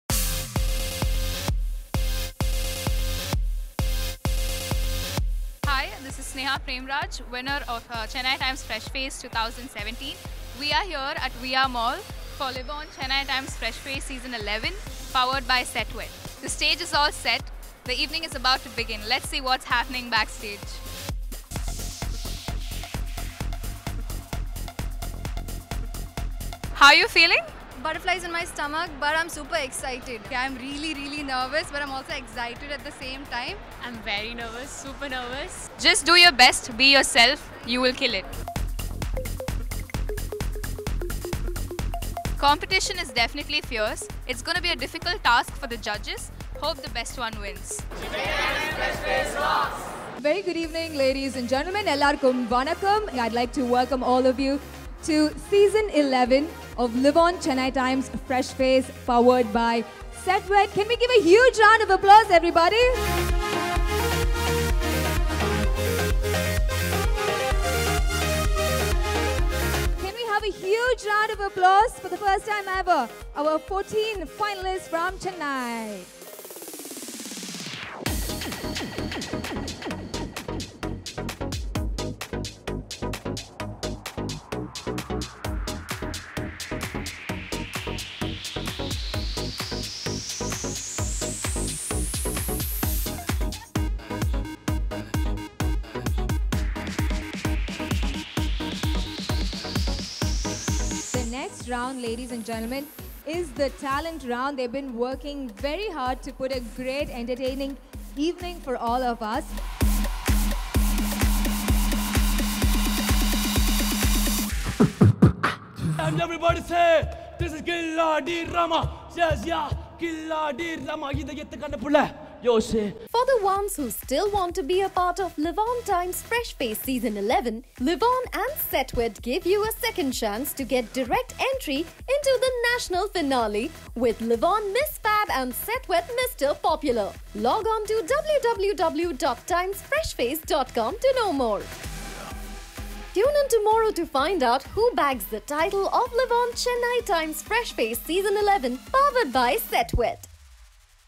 Livon Chennai Times Fresh Face Season 11 reaches its final stage
Livon Chennai Times Fresh Face Season 11 is back in Chennai where it is in its final stage. Contestants showcased their talents for the audience at a local mall.